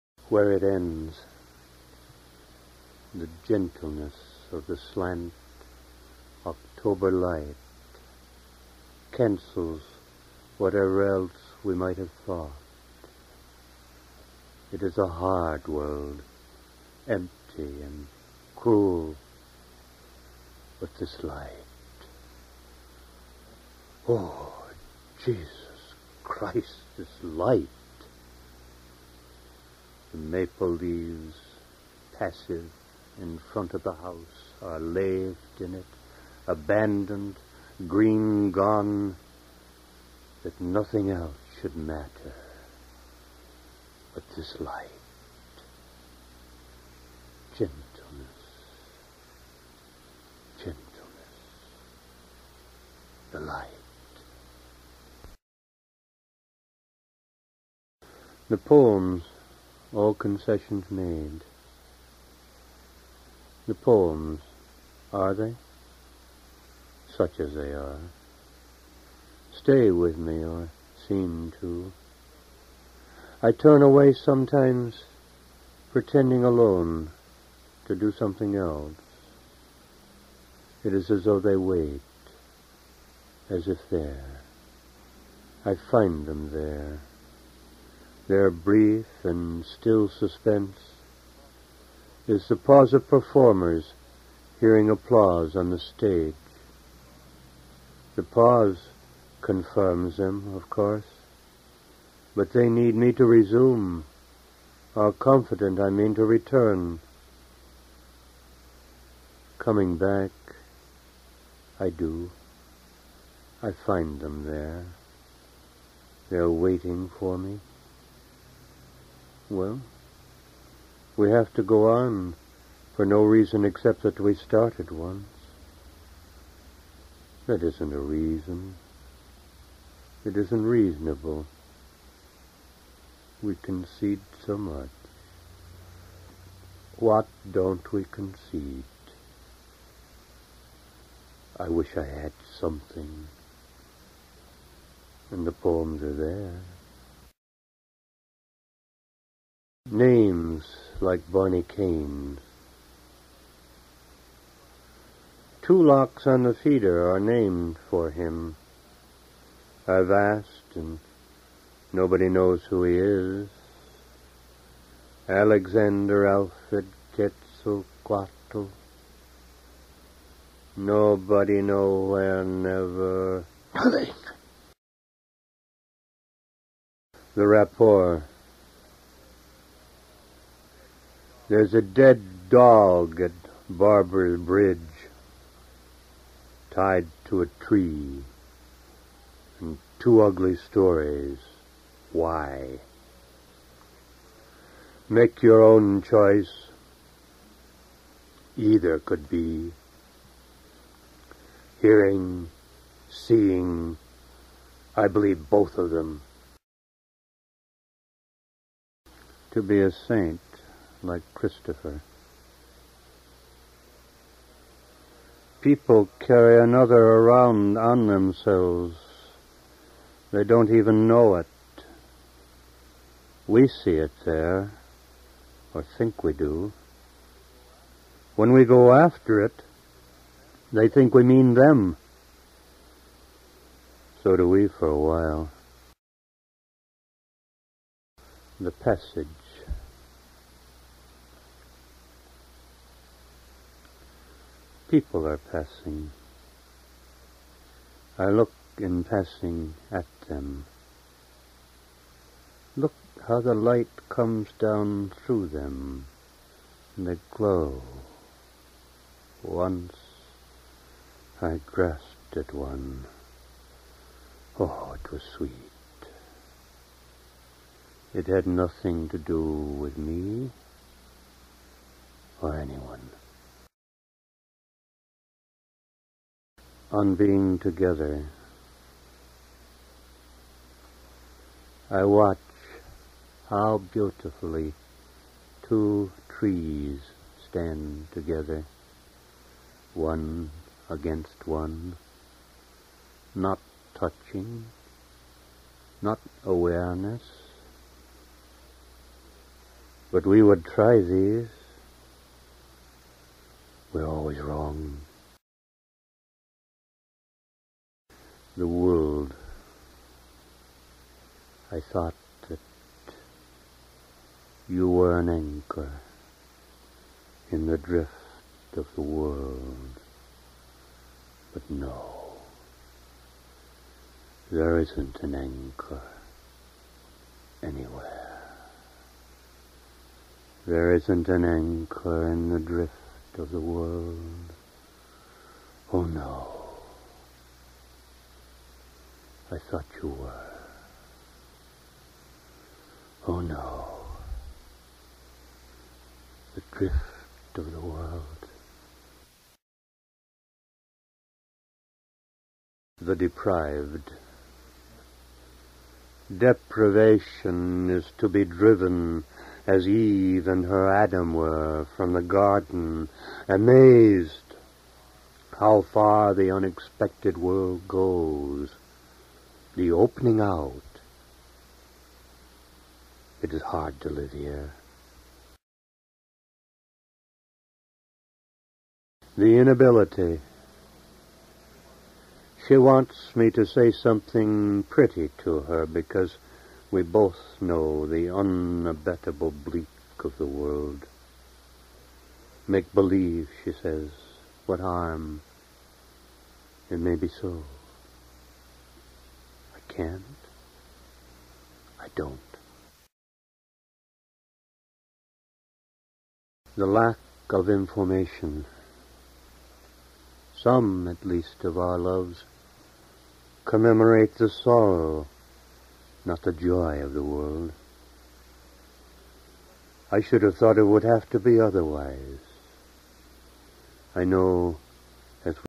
William Bronk poetry reading, 1978 (4/4).
Recorded by Verna Gillis in Hudson Falls, New York on October 13th, 1978.

Poems in this section:

Where It Ends
The Poems: All Concessions Made
Names Like Barney Cain's
The Rapport
To Be a Saint Like Christopher
The Passage
On Being Together
The World
The Deprived
The Inability
The Lack of Information